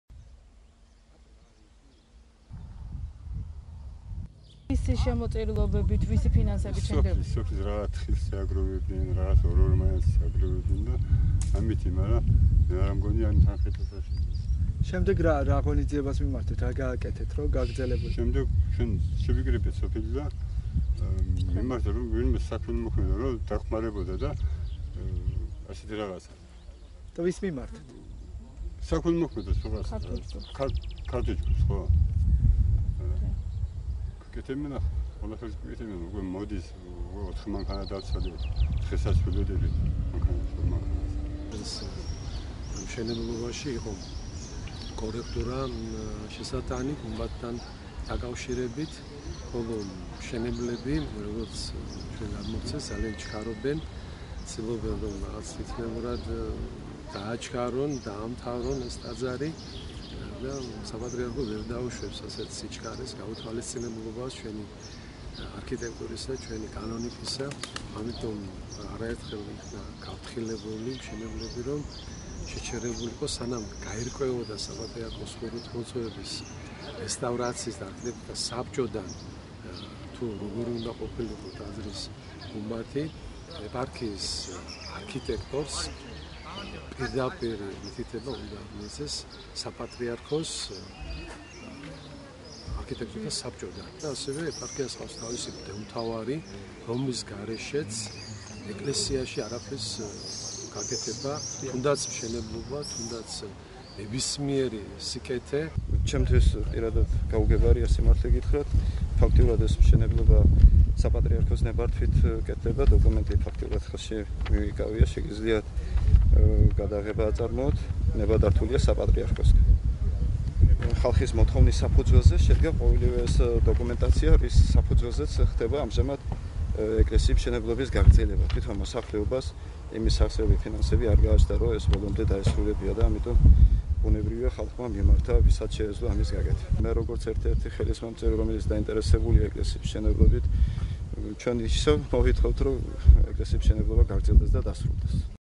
ტაძრის გუმბათი თუ პოლიტიკა
ნარაზენის ტაძარი, რომლის  მშენებლობაც მოსახლეობის შემოწირულობით 11 წელია მიმდინარეობს, შეაჩერეს.